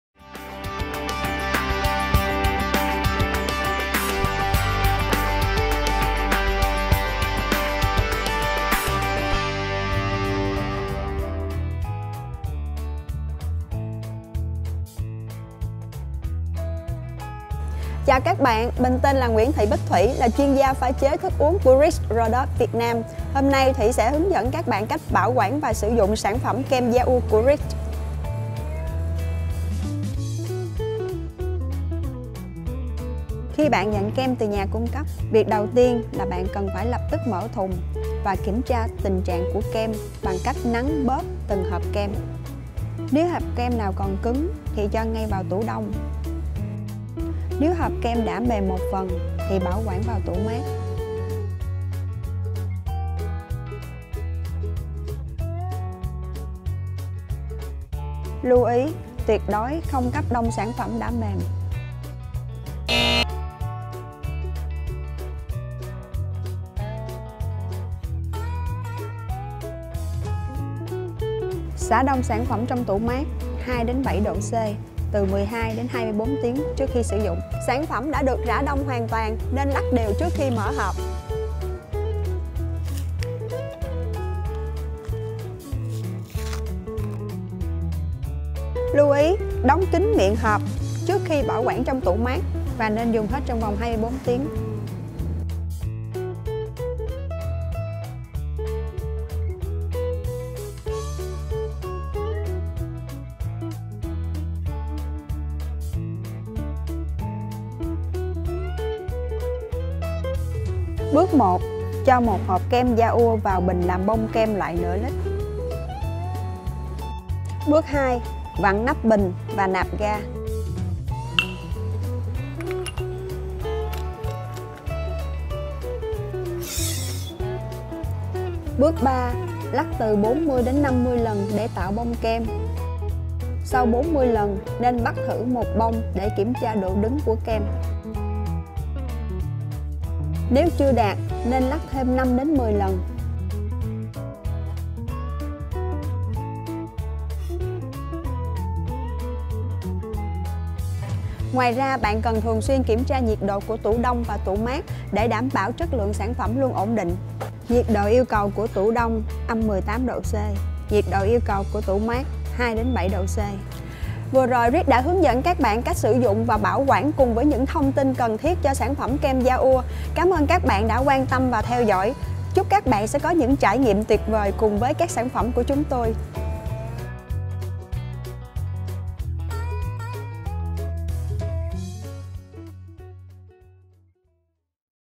Hướng dẫn sử dụng và bảo quản Kem Ya-ua Rich's
Kem Ya-ua của Rich’s là sản phẩm cao cấp đầu tiên và duy nhất tại thị trường Việt Nam, mang hương vị Ya-Ua tự nhiên, giúp tạo nên những thực đơn sáng tạo. Kem Ya-Ua Rich’s là sản phẩm tiện dụng, có thể dùng trực tiếp hoặc đánh bông kem, là nguyên liệu tuyệt vời cho pha chế thức uống và làm bánh.

Đặc tính sản phẩm
Hương vị Ya-Ua béo tự nhiên:
- Hương vị và cấu trúc ổn định nhất trong thời hạn 5 ngày bảo quản với nhiệt độ 2-7o C.
Tính năng linh hoạt :
- Sử dụng làm nguyên liệu cho các món uống và tráng miệng.
- Dễ dàng sử dụng cho ứng dụng bắt bông kem (dollop).
Tính kinh tế cao:
- Sản lượng kem cao, tương đương 300%-350% sau khi đánh bông kem.
Độ ổn định vượt trội:
- Có khả năng kháng axit tốt – có thể trộn hoặc đánh kem cùng với hỗn hợp trái cây có tính chua, kem bơ, phô mai hoặc các nguyên liệu khác.